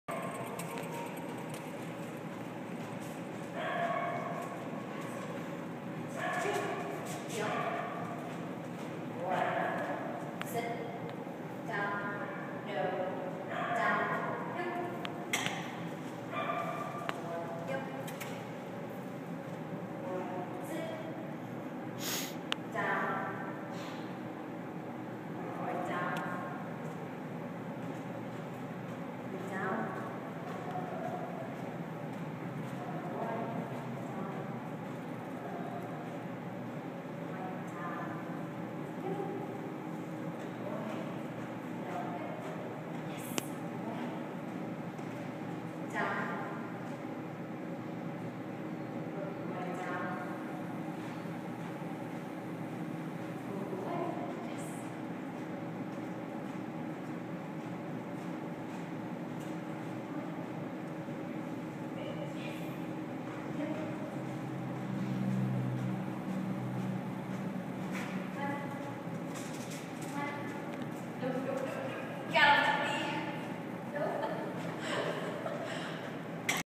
Marley
Marley is working on his sits, downs and stays with the trainer.